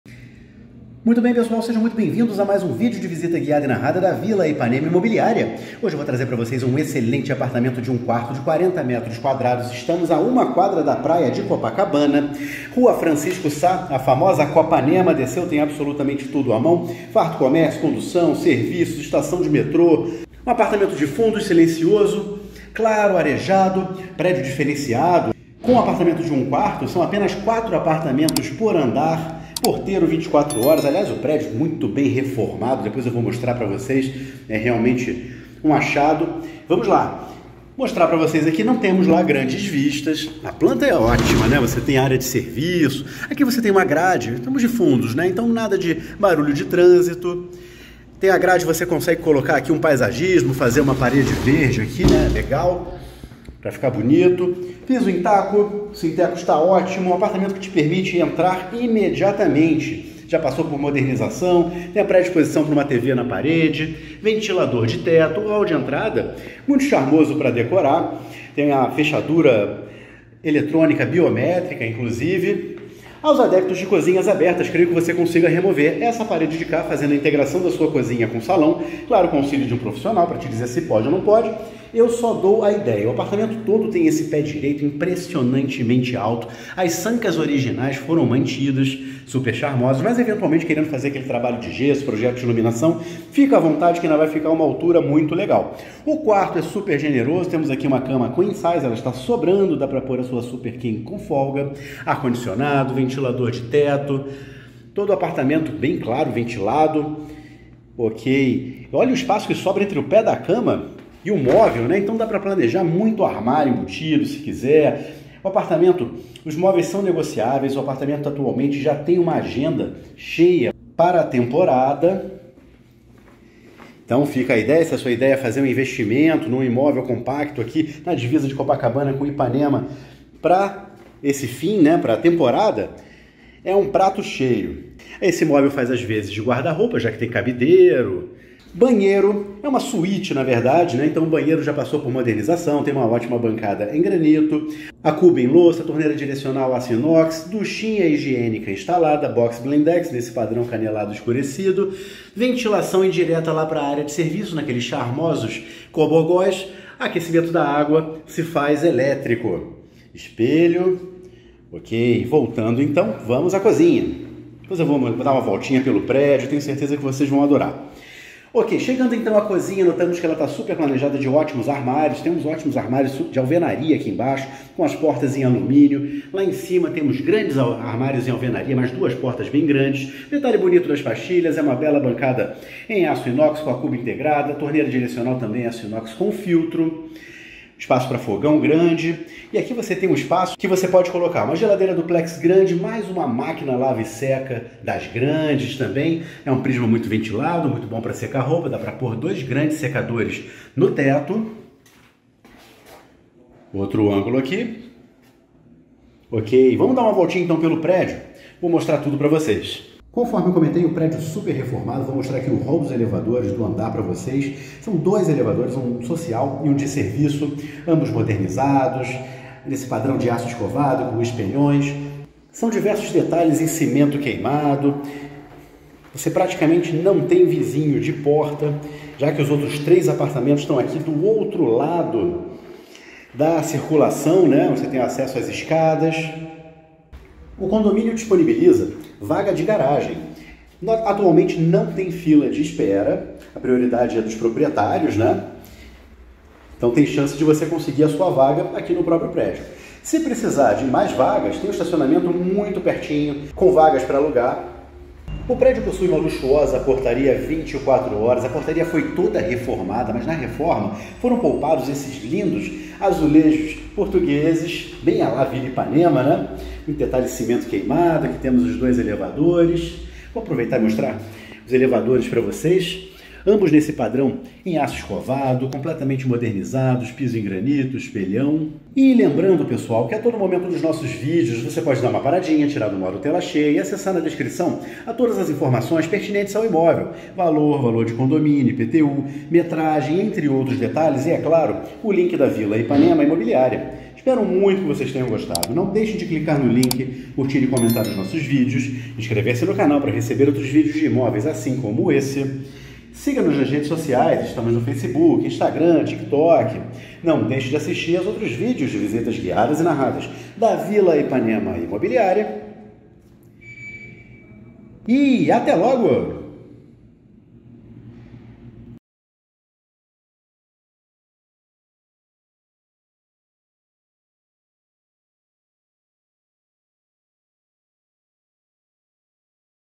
COPACABANA POSTO 6 FRANCISCO SÁ A 1 QUADRA DA PRAIA I SILENCIOSO 40M2 1 QUARTO P.24H OPÇÃO DE VAGA
IMÓVEL VENDIDO!

Valor do imóvel: R$ 540.000,00
Condomínio: R$ 750,00
IPTU: R$ 68,00

*Fazemos a avaliação do seu imóvel gratuitamente. O serviço de filmagem não tem custo adicional!*

Grande oportunidade de excelente apartamento de quarto e sala generoso de 40m2 a 1 quadra da praia no posto 6!

Prédio diferenciado de apenas 4 apartamentos por andar todo reformado, 2 modernos elevadores, luxuosa portaria 24h, câmeras de segurança e vaga de garagem no condomínio.

Pertinho da estação do metrô General Osório (saída Copacabana), arpoador, divisa com Ipanema, farto comércio, condução e serviços.

Sol da manhã, pé direito alto com sancas originais mantidas, peças amplas, fundos, silencioso, claro e arejado.

Salão para 2 ambientes com hall de entrada, ampla suíte, banheiro da suíte modernizado e completo, cozinha grande com área de serviço em anexo destacada cabendo geladeira, fogão e máquina lava e seca roupas grandes.

CJ: RJ - 008630/O